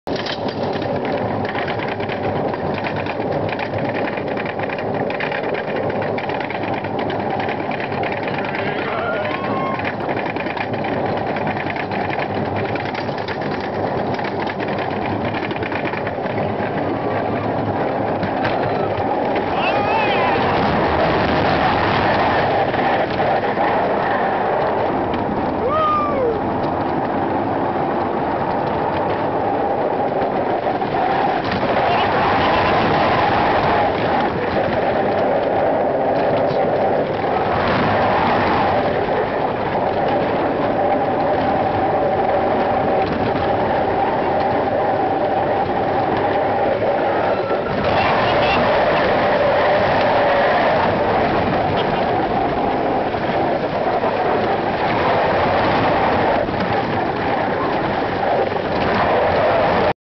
Playland Coaster 1
My buddy Steve and I on the Coaster.